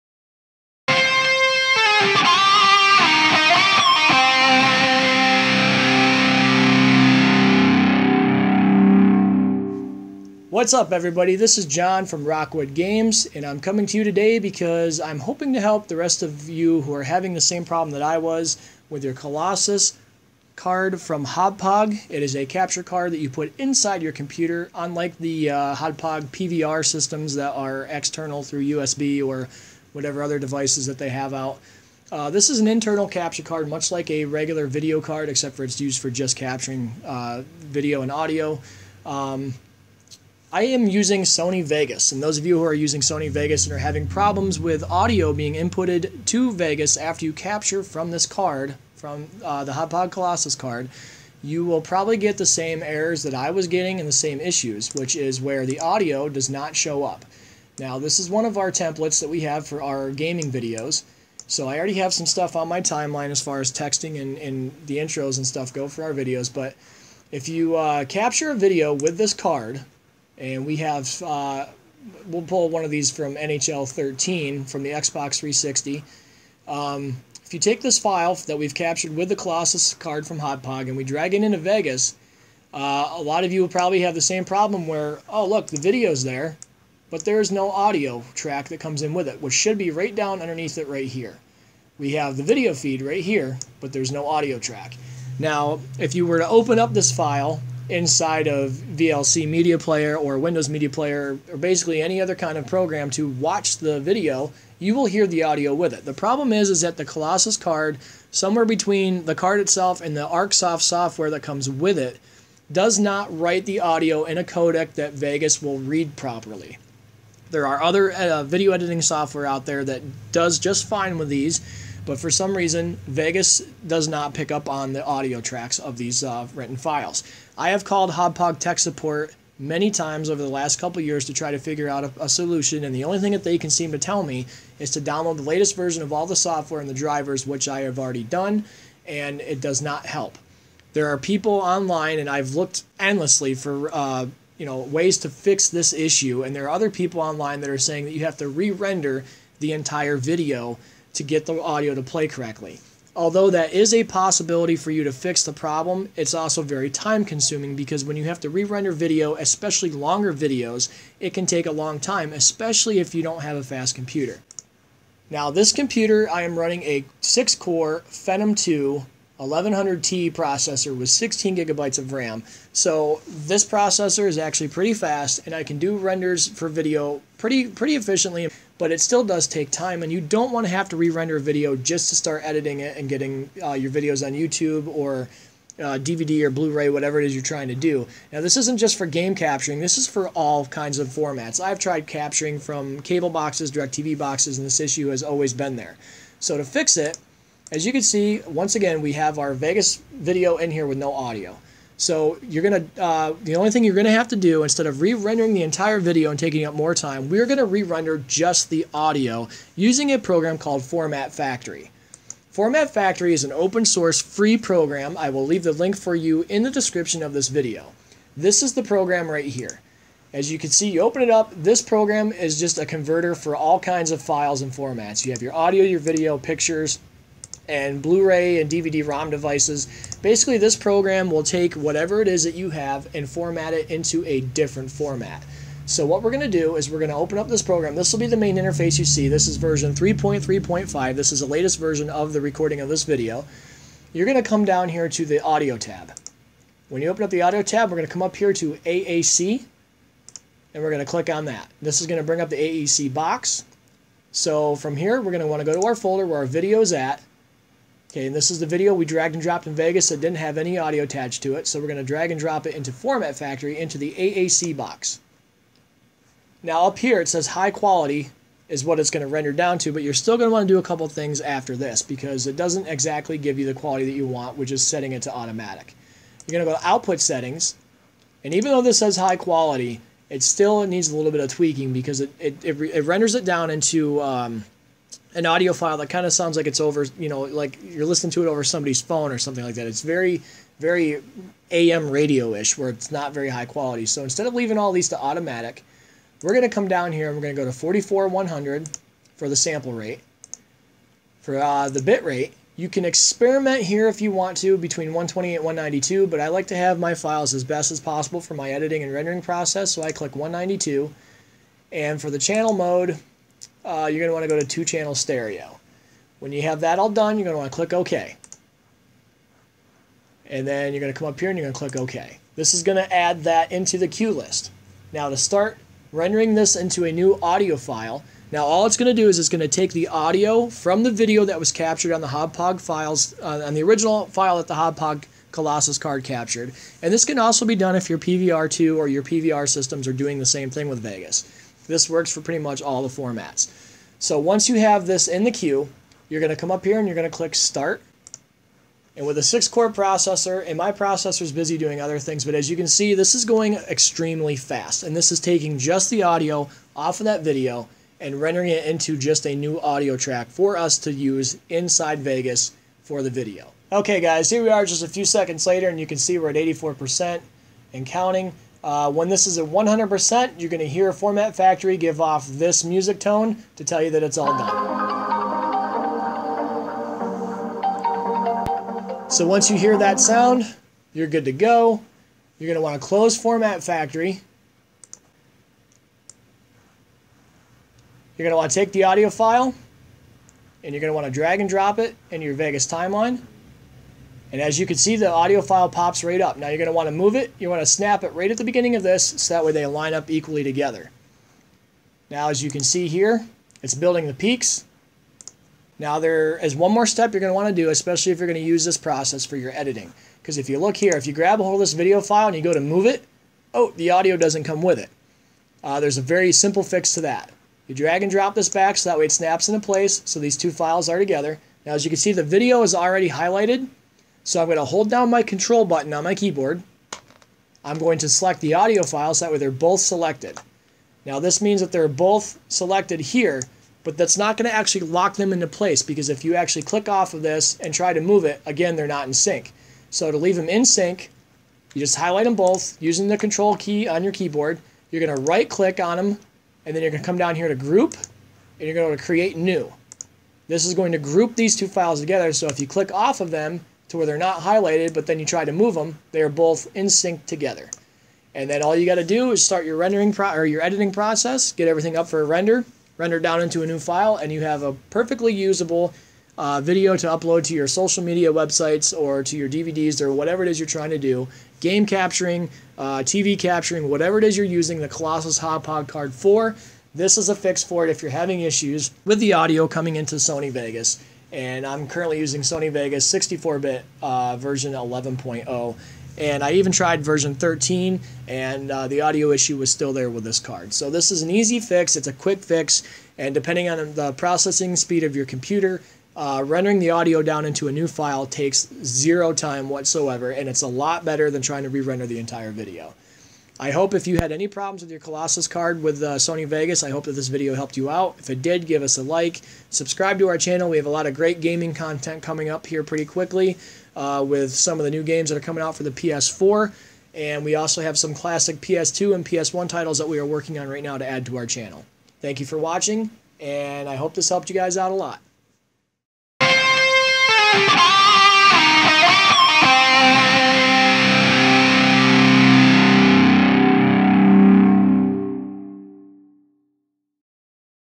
Missing Audio Tracks With Hauppauge Colossus Card in Sony Vegas - FIX - Rockwood Games
This video from Rockwood Games is to help those of you having issues with no audio tracks in Sony Vegas after capturing video from the Happauge Colossus card. Re-gain your audio tracks easy, and fast by rendering new audio tracks in minuets using the software Format Factory.